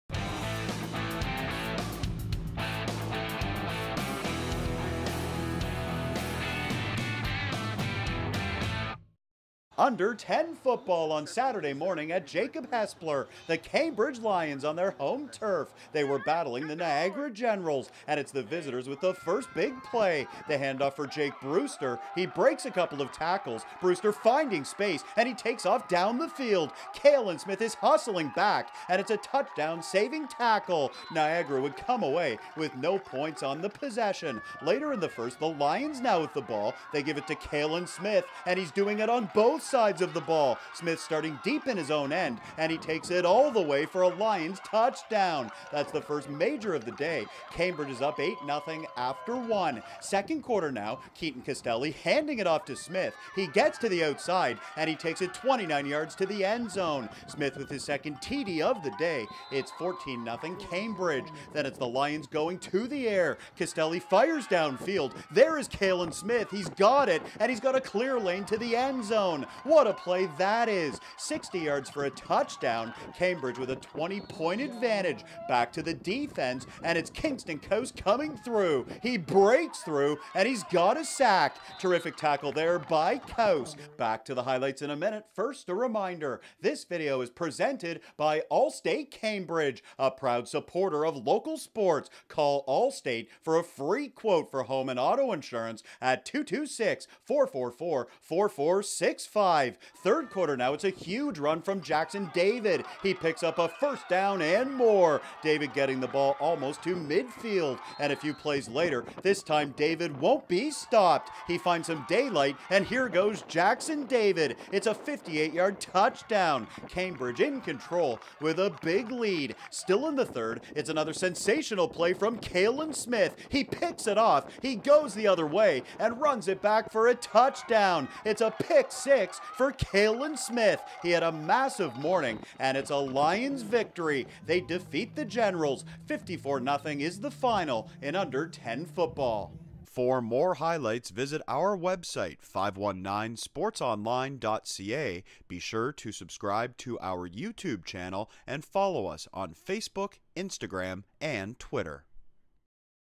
OSFL Under-10 Football - Niagara Generals vs Cambridge Lions
Highlights from Saturday June 4th between the Generals and Lions at Jacob Hespeler Secondary School.

Call Allstate for a free quote for home and auto insurance.